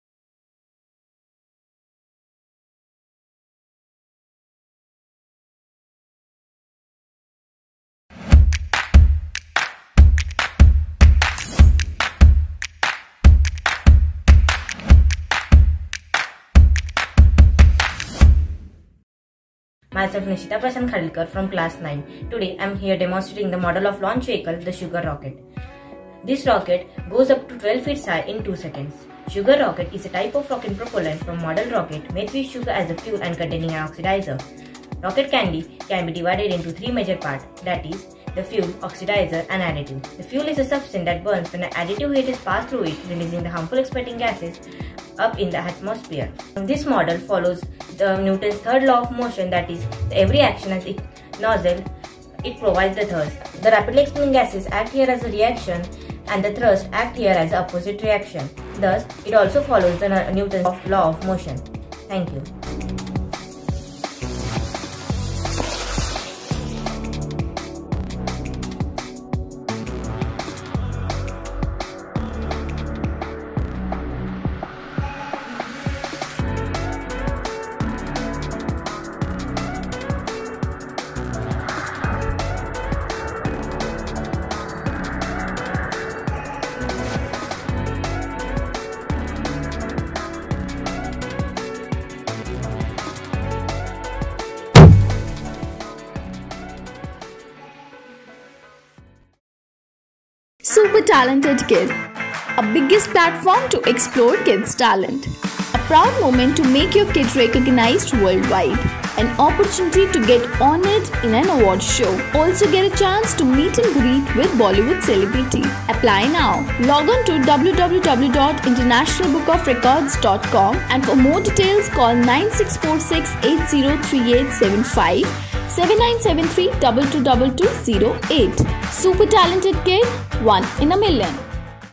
YOUNGEST TO MADE AND LAUNCH SUGAR ROCKET
The World Record of “YOUNGEST TO MAKE AND LAUNCH SUGAR ROCKET” is achieved by NISHITA KHADILKAR on 3rd January 2021 from Chandrapur (Maharashtra) India. 14 years 15 days old Nishita made and launched sugar Rocket. She used sugar as fuel and KNO3 Oxidizer to make and launch it and has set a new world record for International Book of Records.